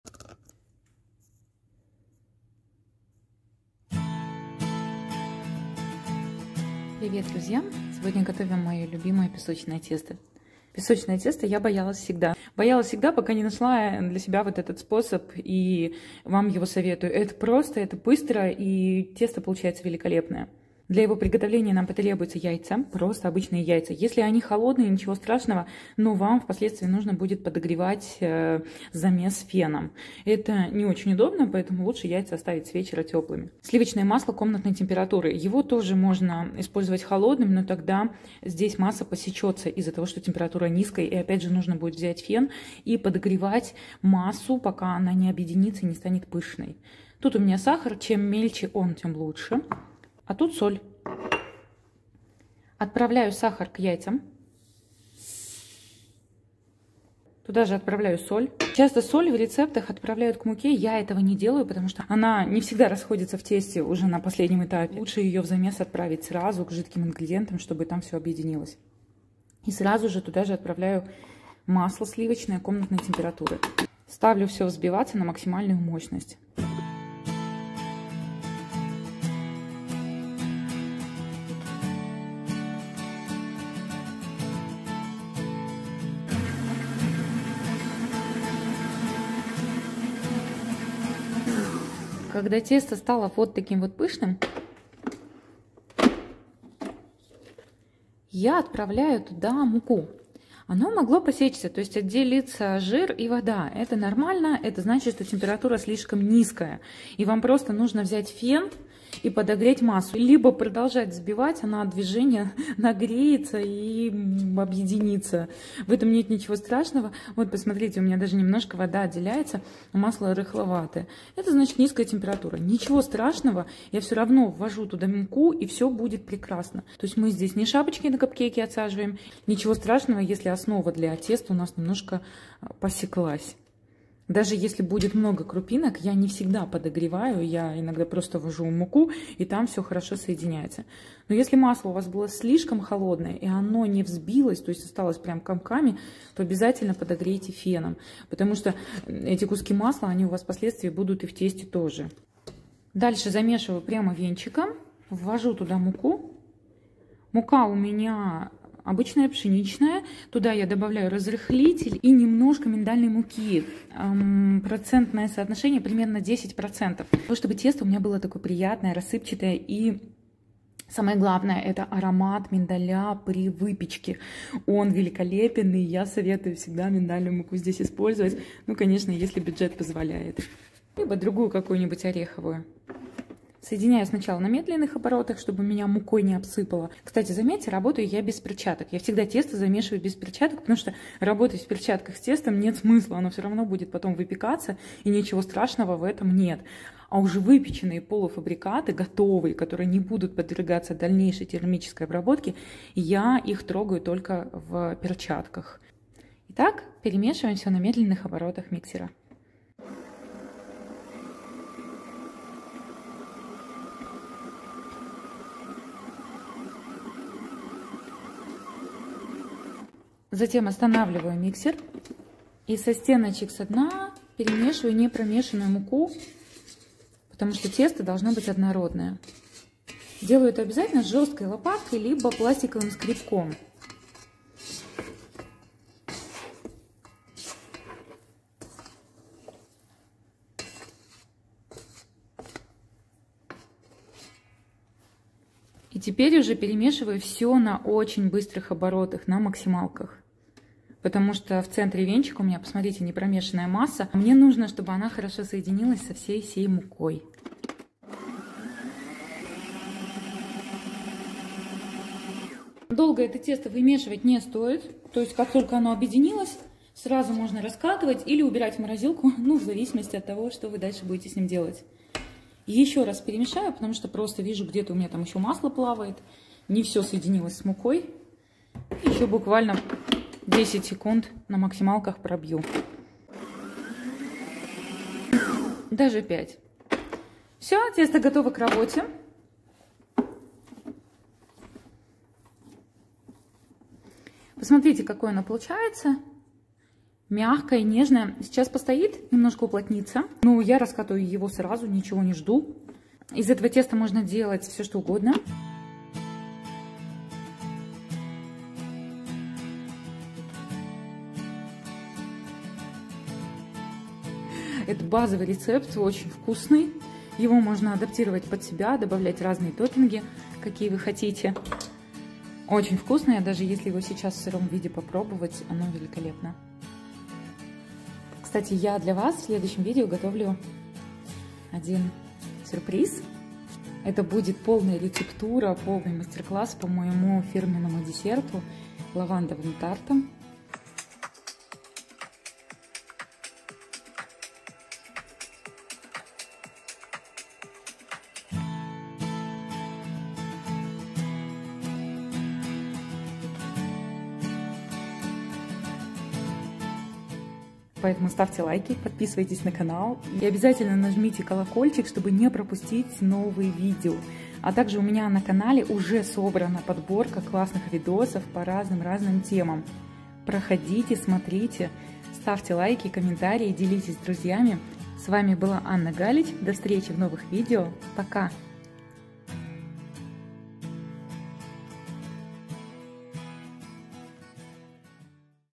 Идеальное песочное тесто ❤ Рецепт песочного теста для пирога/печенья.
Это мой любимый рецепт песочного теста! Он очень простой и замешивается из теплого сливочного масла и не руками, а миксером!

В видео я пошагово покажу рецепт из которого вы сможете приготовить как печенье, так и пироги, тарталетки, тарты. Тесто очень воздушное и нежное. Легко готовится в домашних условиях!

Хранить готовое тесто можно в плотной упаковке в холодильнике до 3-х дней или в морозильной камере до 3-х месяцев.

Если у вас нет миндальной муки, то замените ее на цельнозерновую пшеничную - это придаст интересную текстуру тесту. Если и такой муки нет, то на обычную пшеничную.

В следующих видео я покажу как можно из этого песочного теста приготовить основу для тарта и заполнить нежнейшим лавандовым муссом.

Благодарю за внимание и комментарии! Спасибо что делитесь моими видео с друзьями, тем самым вносите вклад в развитие канала.

Пеките, наслаждайтесь!
С уважением, Анна Галич.